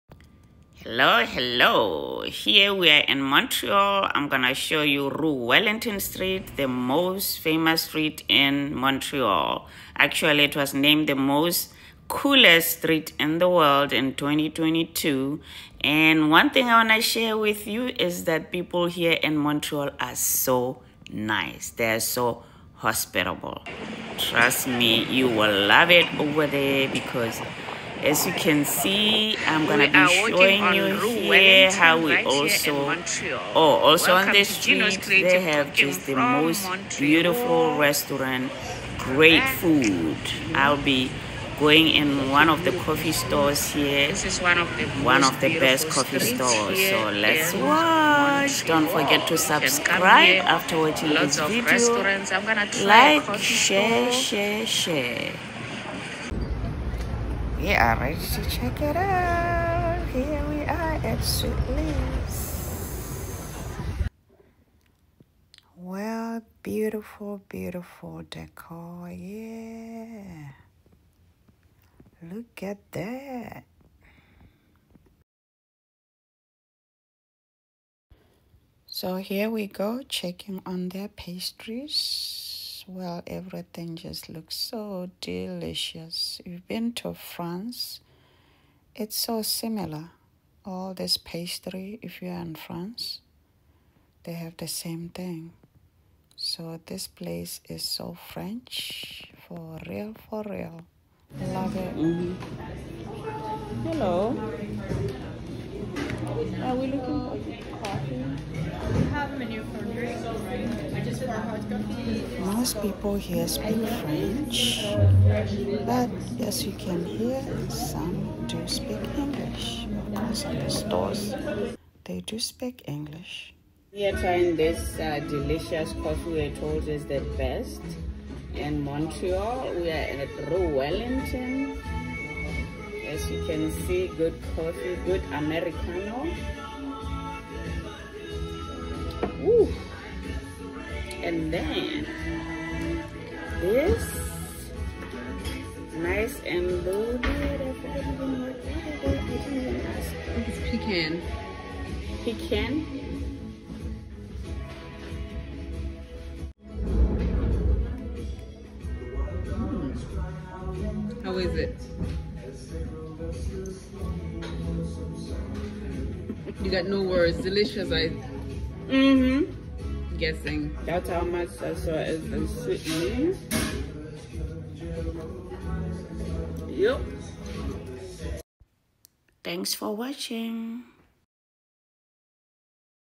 Checkout The Famous Coolest Street In The World! | Rue Wellington Street In Montreal Canada
checkout the famous coolest Street in the world, right in Montreal, Quebec; Canada. Rue Wellington Street was named the coolest street in the World in 2022. The street is lined with the best restaurants, bakeries. We went into one of the bakery cafe called Sweet Lee’s, coffee and pastry was top notch!😋😋. Montreal has the most nicest people you can ever come across! I will be back!